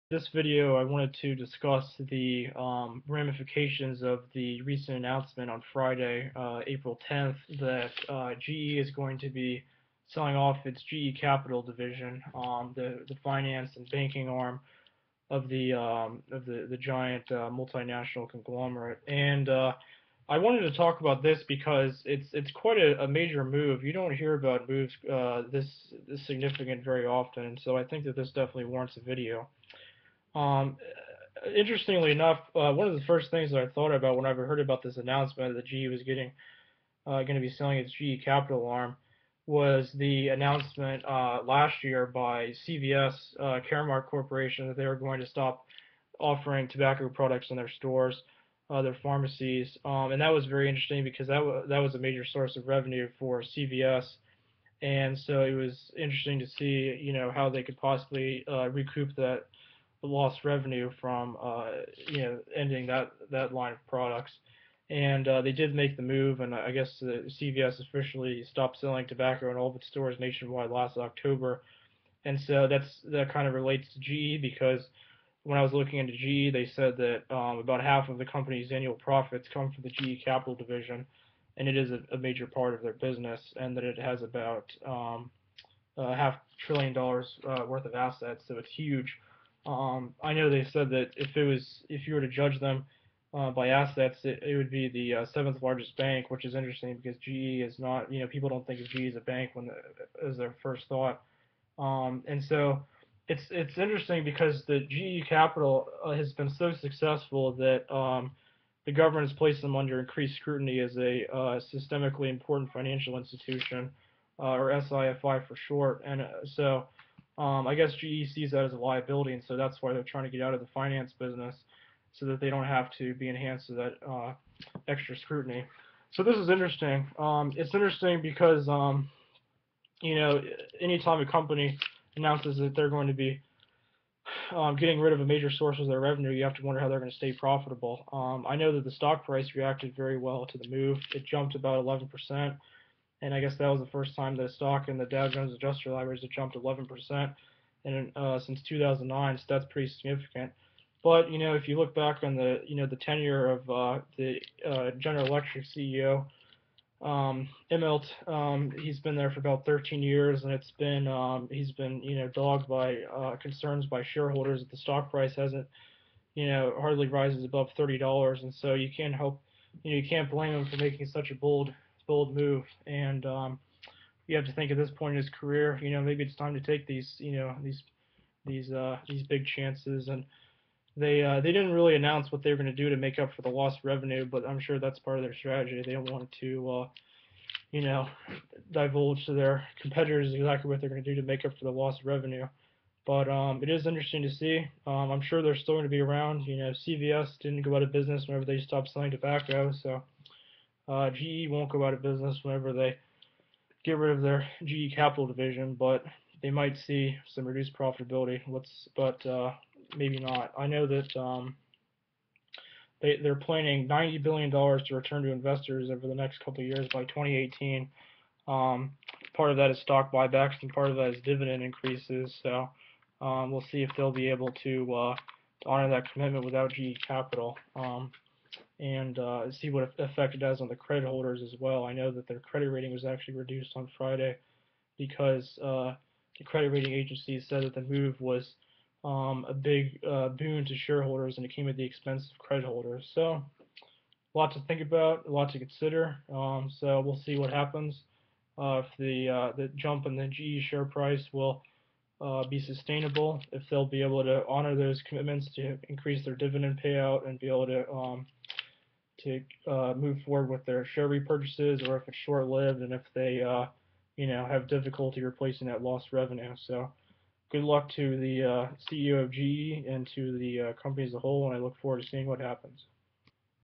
How Will General Electric Fare Without GE Capital?
Following the April 10th announcement that GE will be selling off it's GE Capital arm, many investors are wondering, how will they replace that revenue?  I offer my opinion on whether the move will pay off and what it means for the companies future.